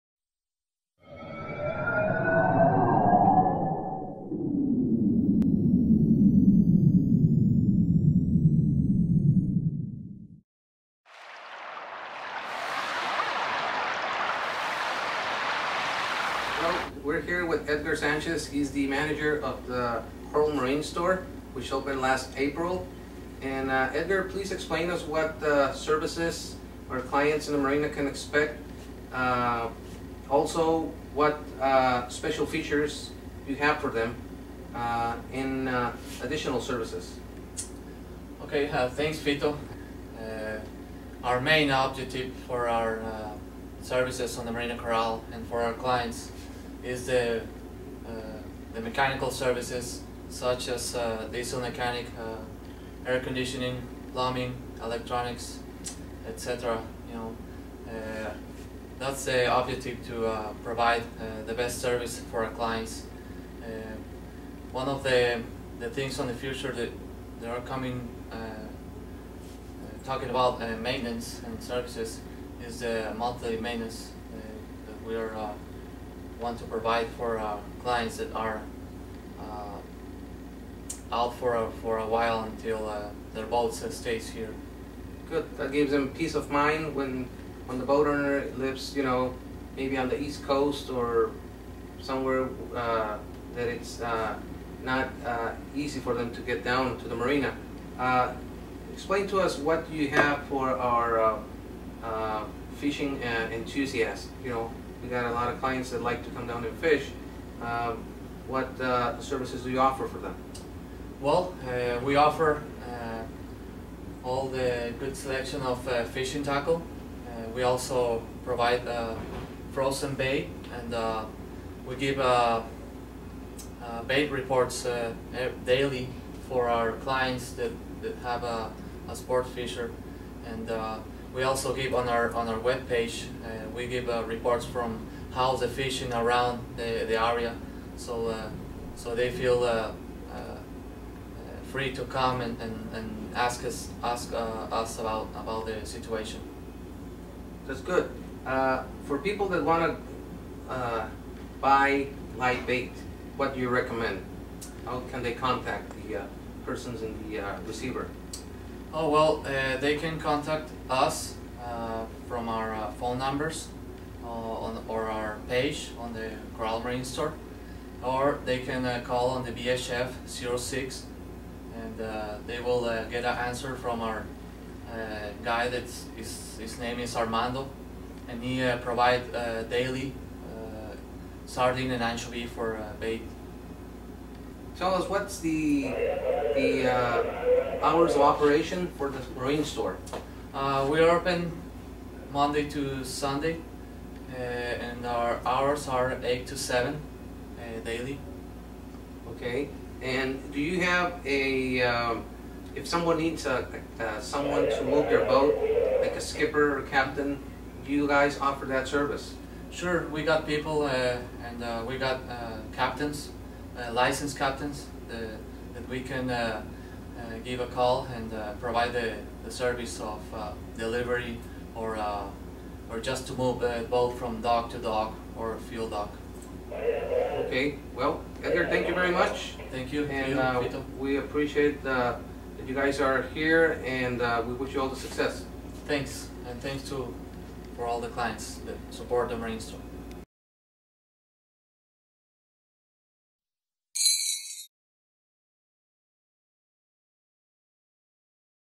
Coral Marina Store, Ensenada Baja California | Hotel Coral & Marina
Hotel Coral & Marina a través de Coral Marine Store ofrece una amplia gama de productos y servicios mecánicos para los amantes del mar, marineros y dueños de yates.
Visita Ensenada, Baja California.

Hotel Coral & Marina by its Coral Marine Store offers a wide variety of products and mechanical services for the sea lovers, mariners and yacht owners.
Visit Ensenada, Baja California.